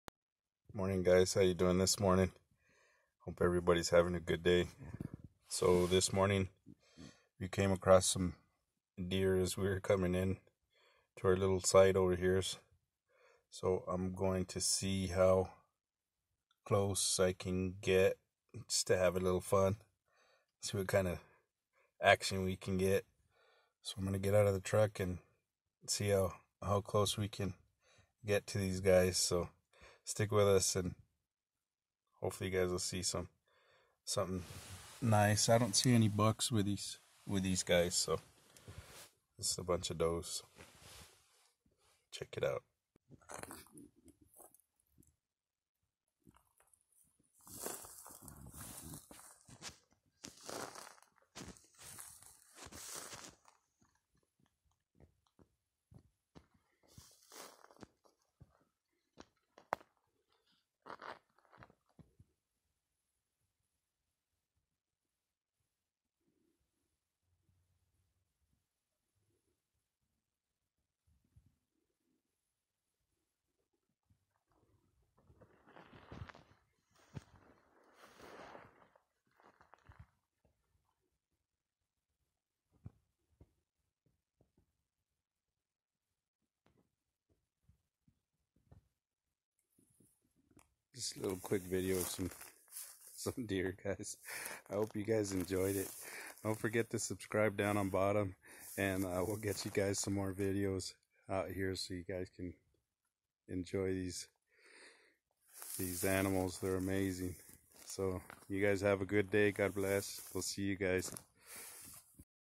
Traditional archery ( a little fun)
Join me as I stalk a couple of doe for some practice.